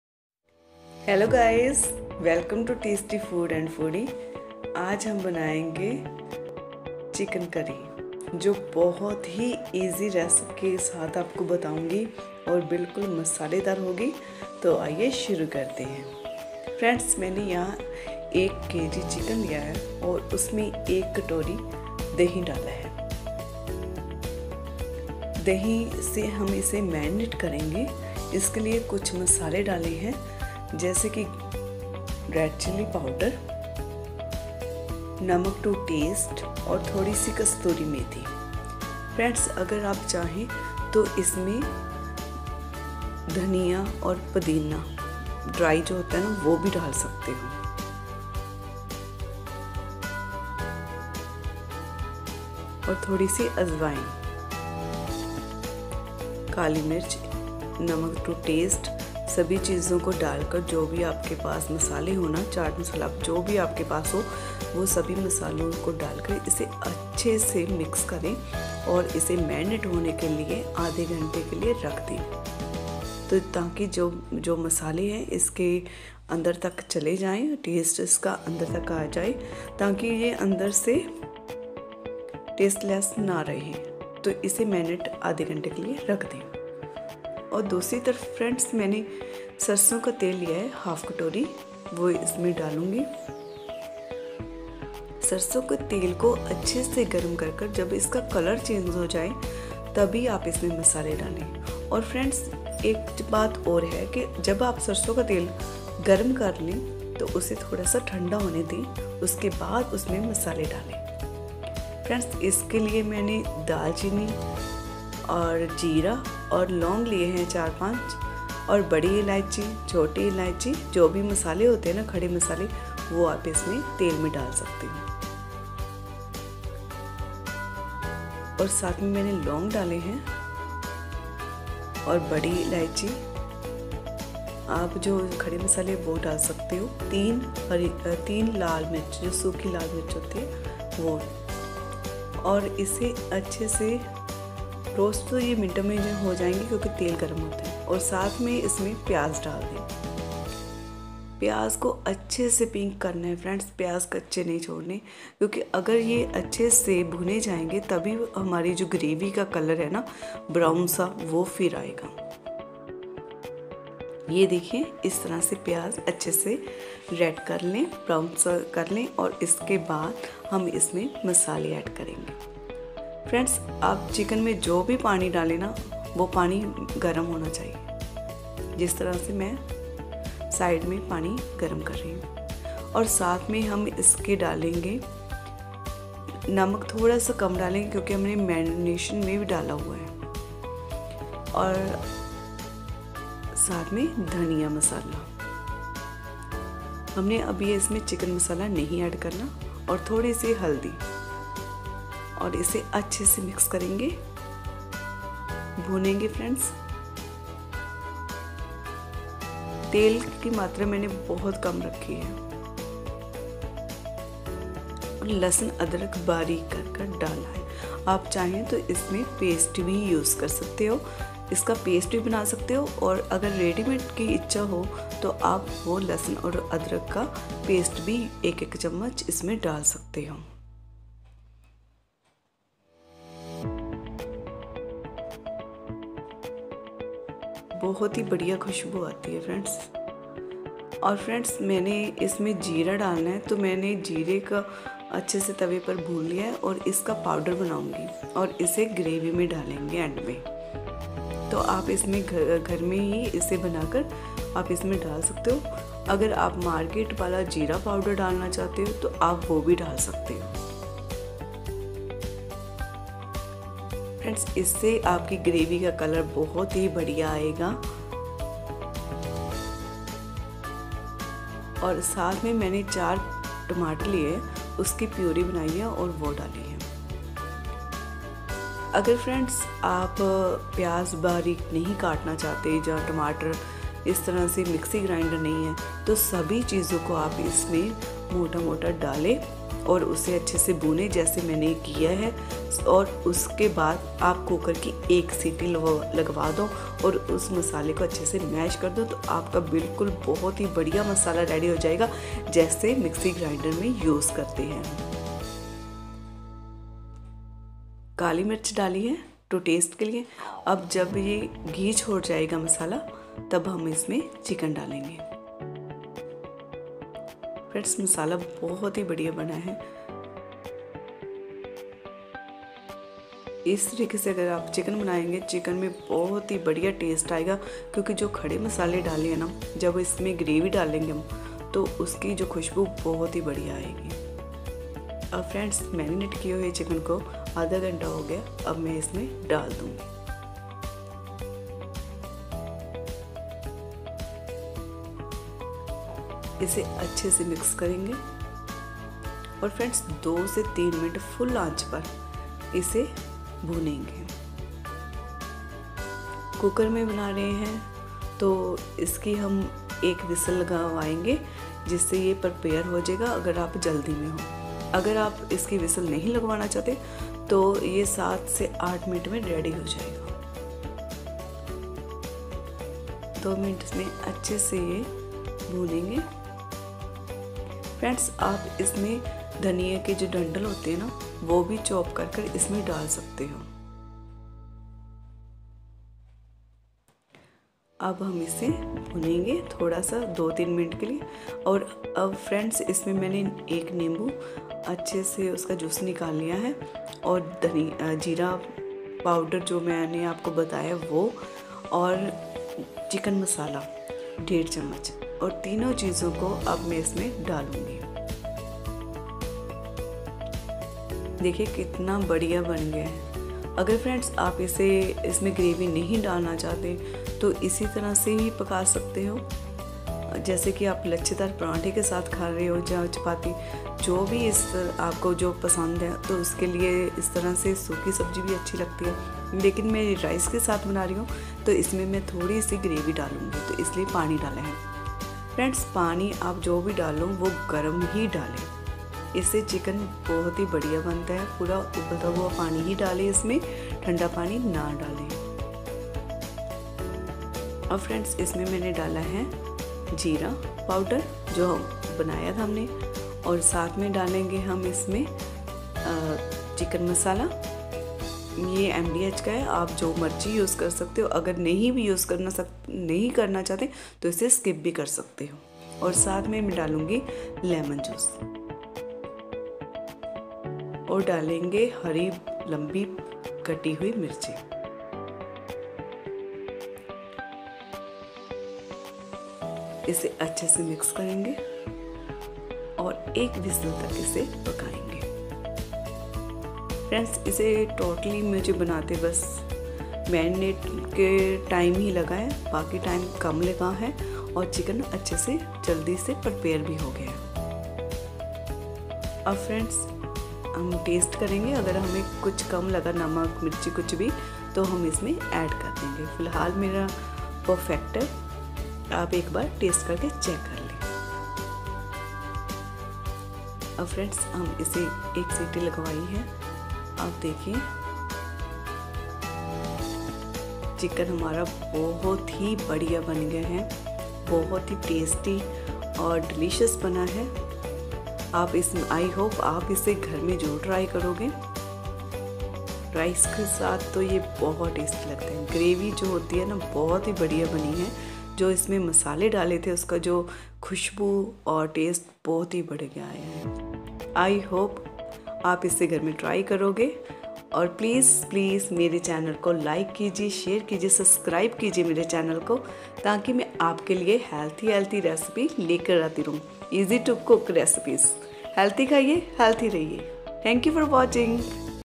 Masala Chicken Curry|Masaledaar Chicken Curry At Home|Restaurant Style
Chicken Curry Recipe is very delicious and healthy. This Recipe is special for Nonveg Lovers who Love to eat new recipe of chicken. Moreover this chicken Curry is not only tasty but also healthy in winter to make you safe in this cold.

Preparation Time :
Marinate- 30 Min
Cooking- 20 Min

Team:Tasty Food And Foodie